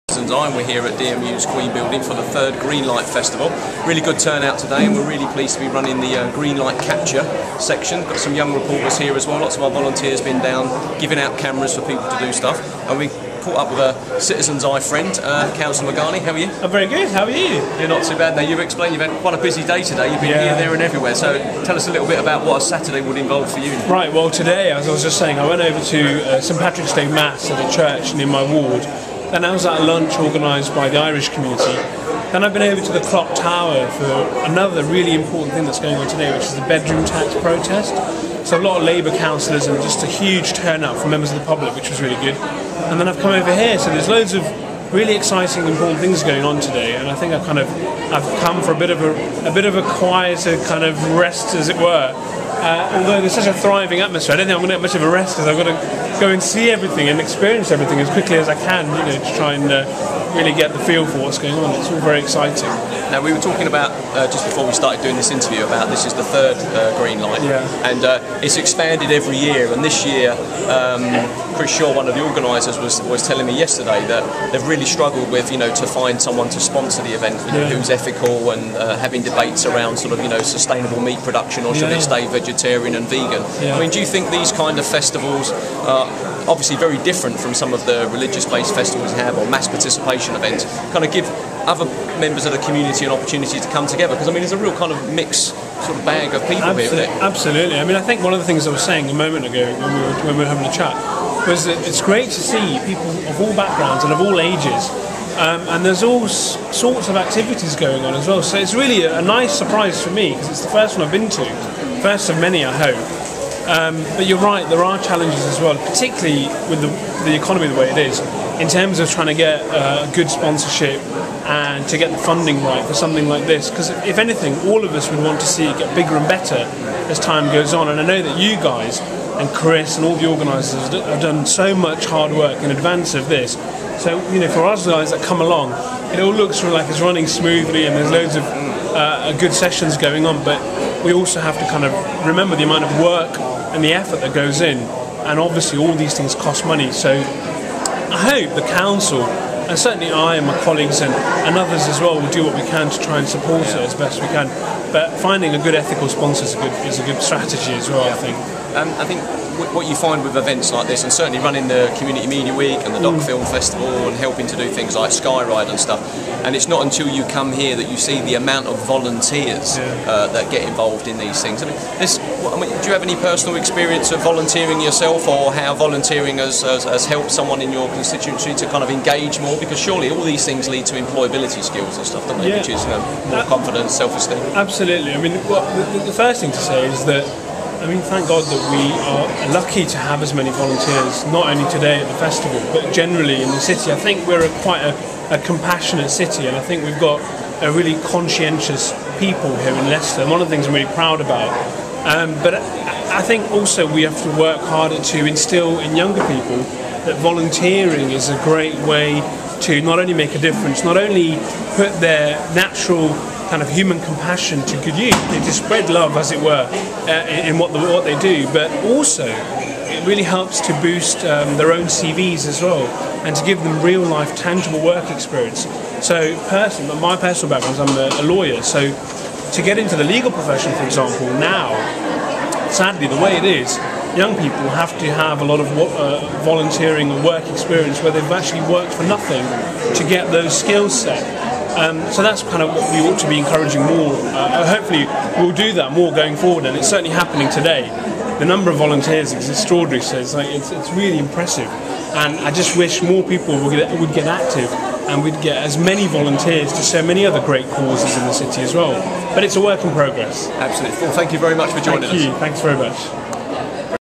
Greenlight Festival 2013
Interview with Cllr Medhani at the Greenlight Festival 2013 today.